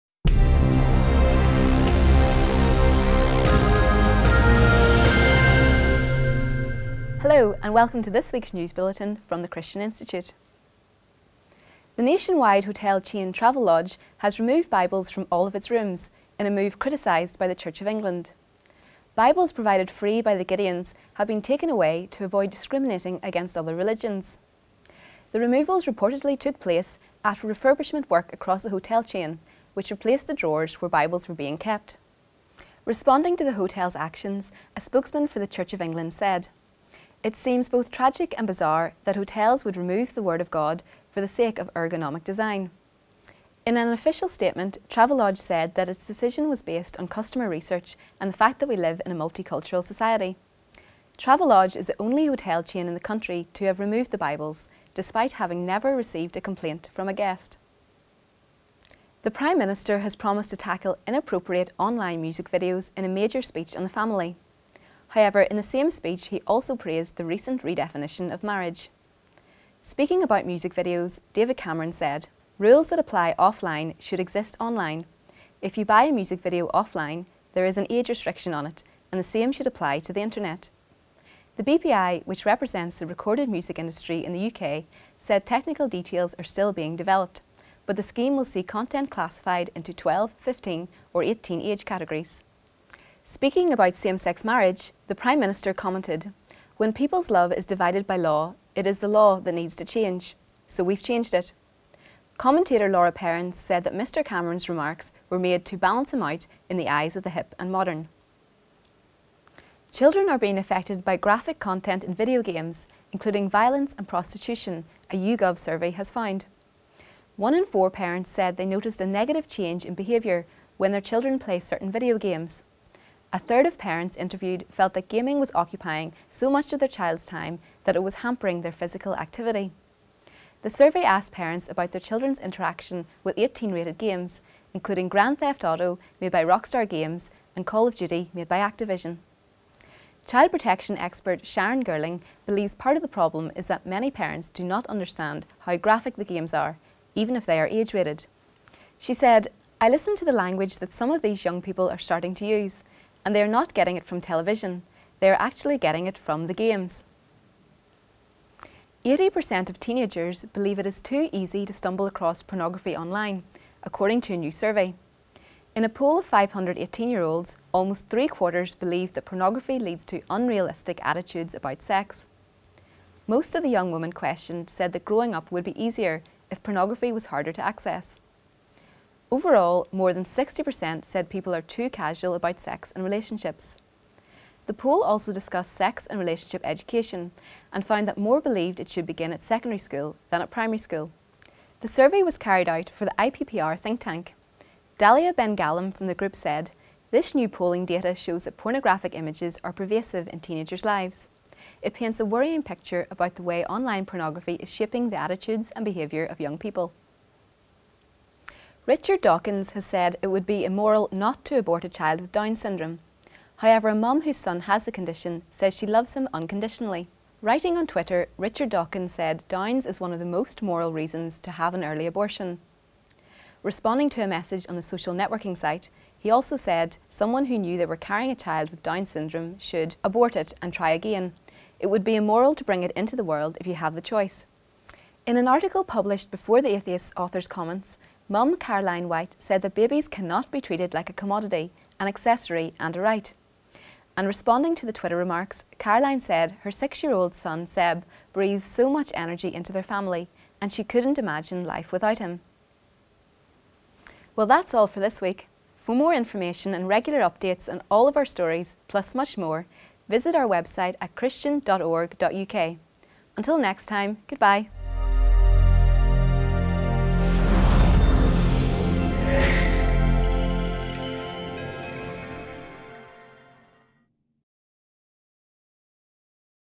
News Bulletin 22 August 2014 -- The Christian Institute
Nationwide hotel chain Travelodge removes Gideons Bibles from all of its rooms -- The Prime Minister promises to tackle inappropriate online music videos and praises same-sex marriage in a major speech on the family – A YouGov survey finds that children are being affected by graphic content in video games, including violence and prostitution – A new poll shows that 80 per cent of teenagers believe it is too easy to stumble across pornography online – Atheist Richard Dawkins says it would be immoral not to abort a child with Down’s syndrome.